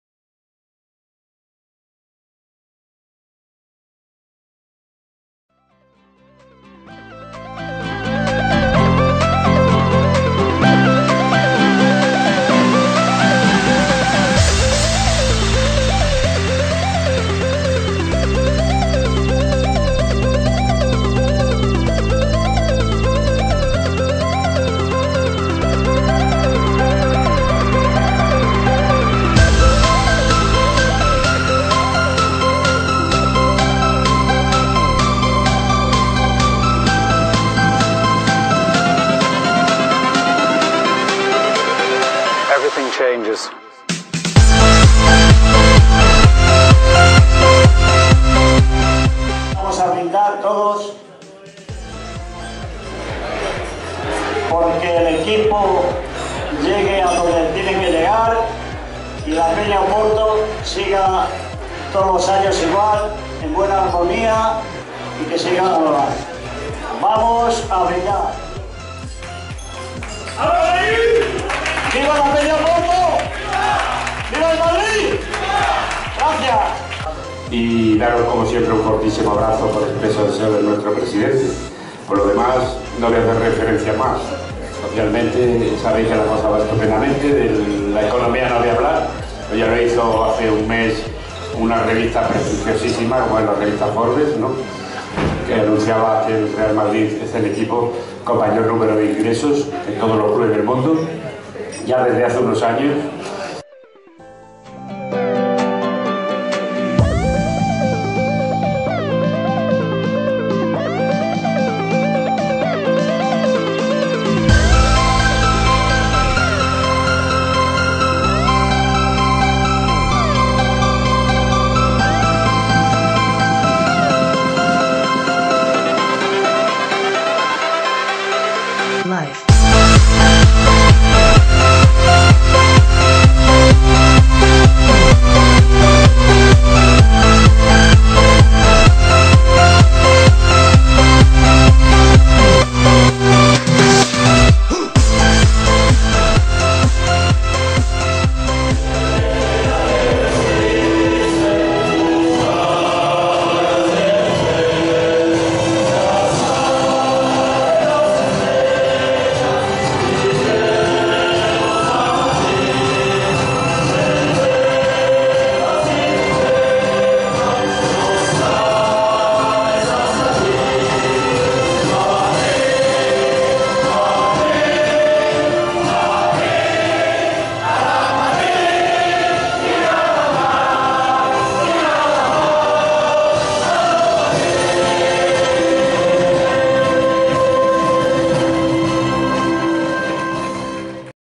PM Oporto 55º aniversario
La Peña Madridista “Oporto “(Madrid) celebró su 55º aniversario. Al acto asistió el exjugador José Luis López Peinado en representación del club blanco. Los miembros de la Peña Madridista  disfrutaron de un día inolvidable.